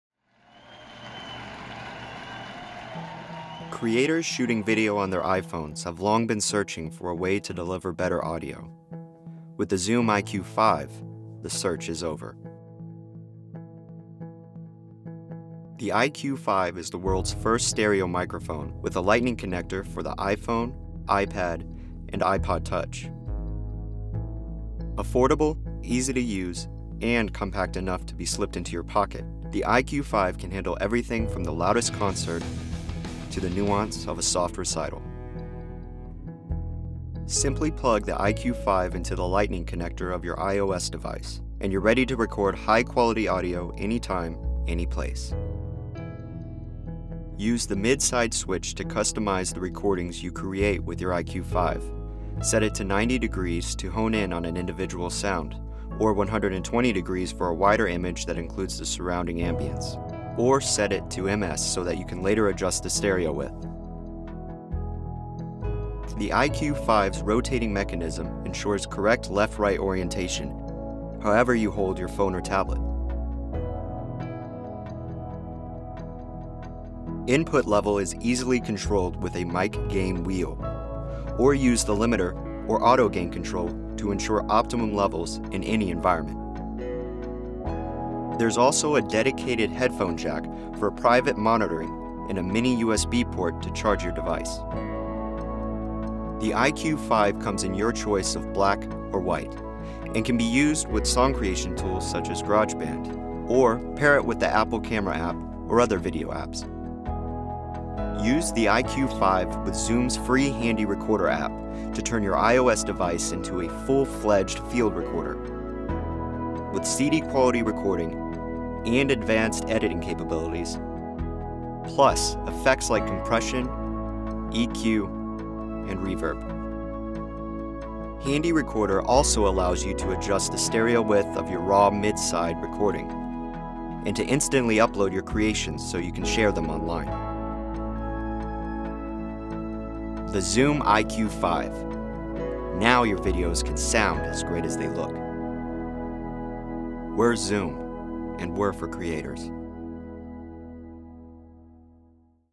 Zoom iQ5 Product Video
The first stereo microphone for lightning enabled for the iPhone, iPad and iPod Touch.